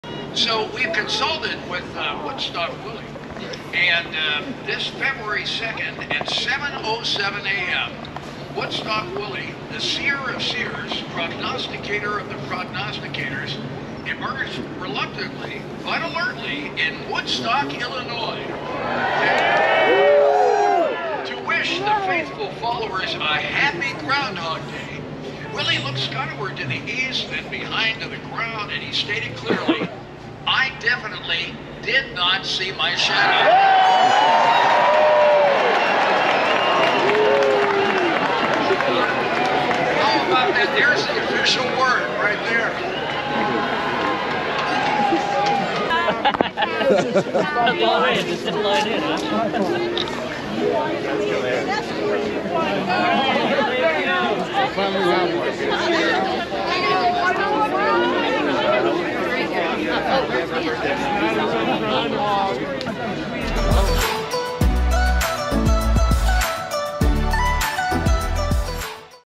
The Police Escort Tom Skilling As He Joins Us in Woodstock, IL For Groundhogs Day
Join TomSkilling in Woodstock for Groundhogs day 2024
Happy Retirement Tom!